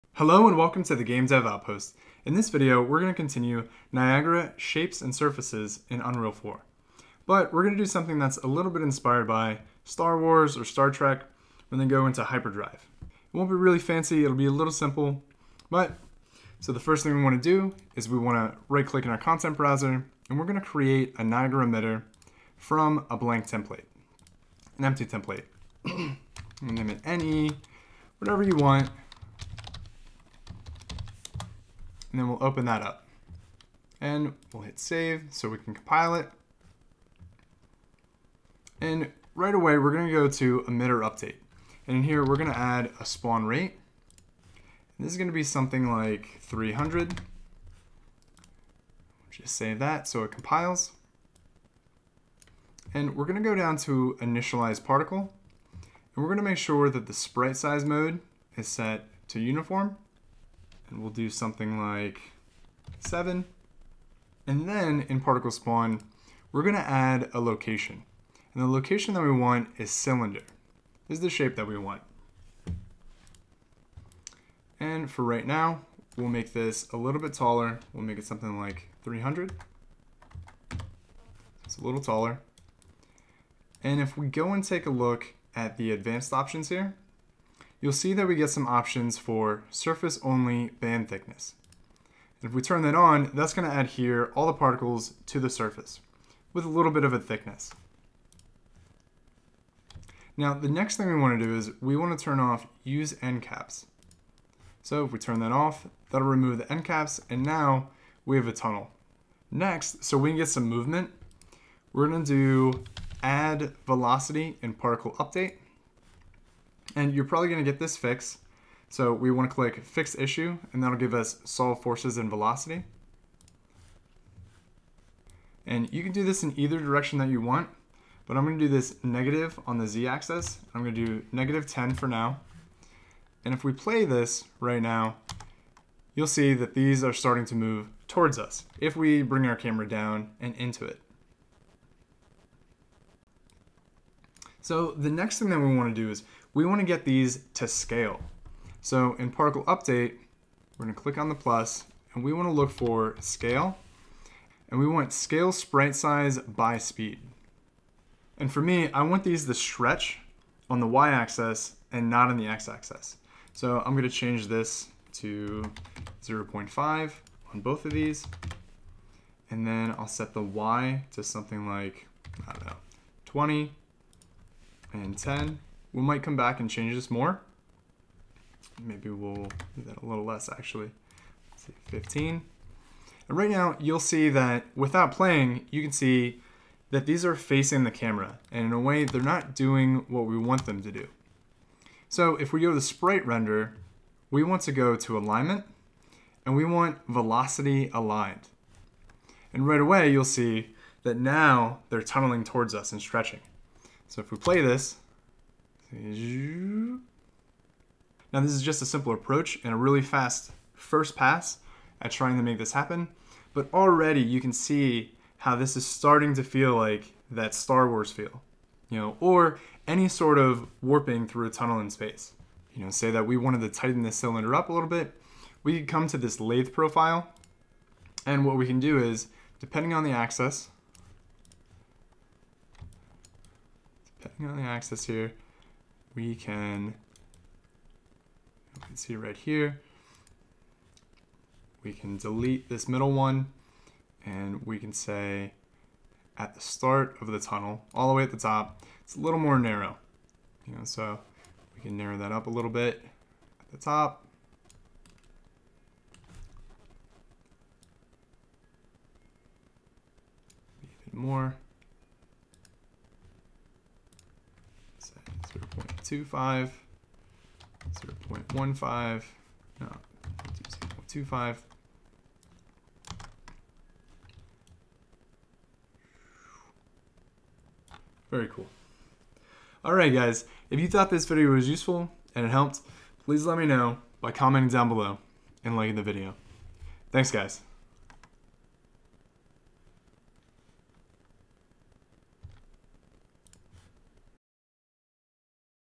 UE4 - Niagara Shapes and Surface - Part 2 - Star Wars
In this video we'll talk about Shapes and Surfaces for Niagara in UE4. We'll create a simple Starwars inspired warp tunnel.